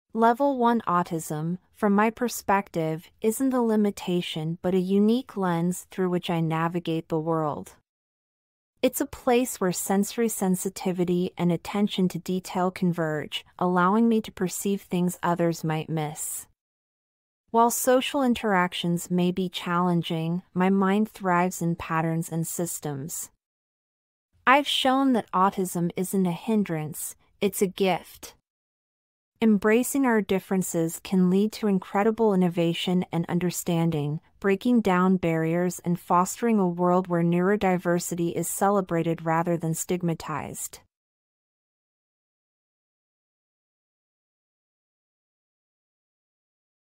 Temple Grandin About Level 1 Autism
In today's video, we explore the world of Level 1 Autism through the imaginary point of view of the renowned autism advocate and author, Dr. Temple Grandin. 🌟

Join us as we delve into the unique insights and perspectives that Dr. Grandin might share regarding Level 1 Autism, often referred to as "high-functioning autism" or "Asperger's syndrome." Dr. Temple Grandin, herself on the autism spectrum, offers a valuable viewpoint on this topic that can help us better understand and appreciate the experiences of individuals with Level 1 Autism.